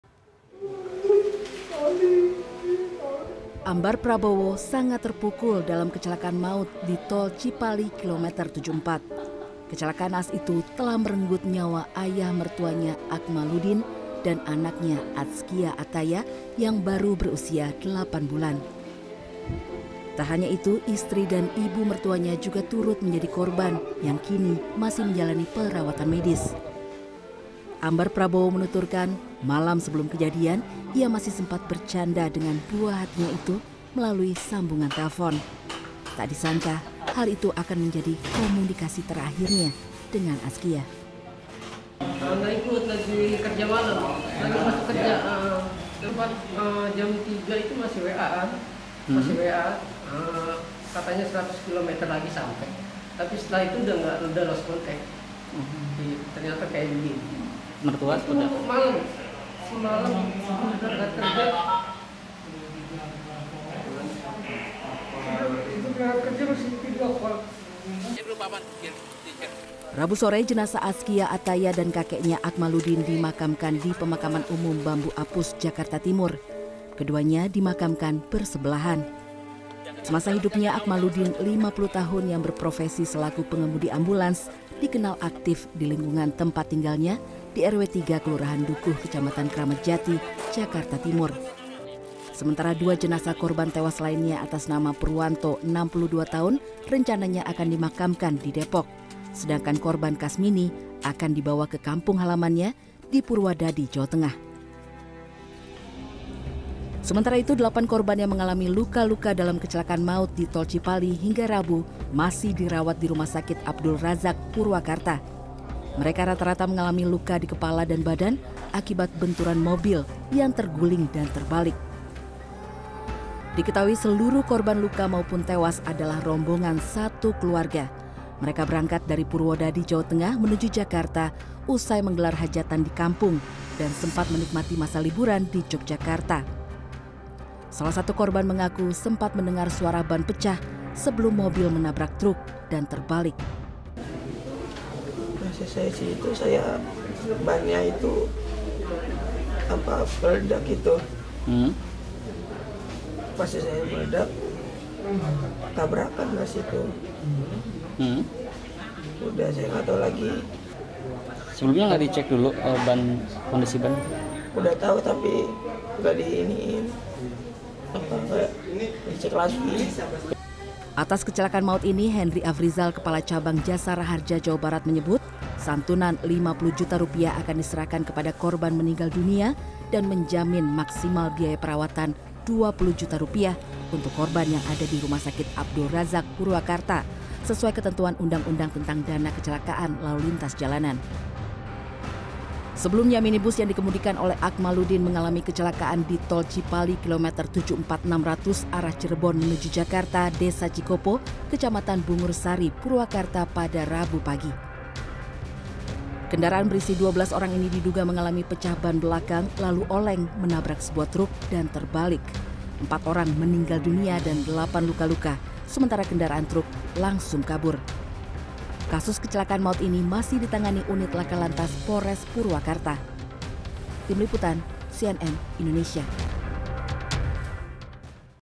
Kecelakaan Maut Satu Keluarga di Tol Cipali
Empat korban tewas dalam kecelakaan maut di Tol Cipali , kilometer 74 arah Jakarta dibawa pulang keluarganya.

Dua diantaranya, yang merupakan kakek dan cucu, telah dimakamkan bersebelahan di TPU Bambu Apus Jakarta Timur.

Sementara 8 korban luka, masih menjalani perawatan di Rumah Sakit Abdul Radjak, Purwakarta.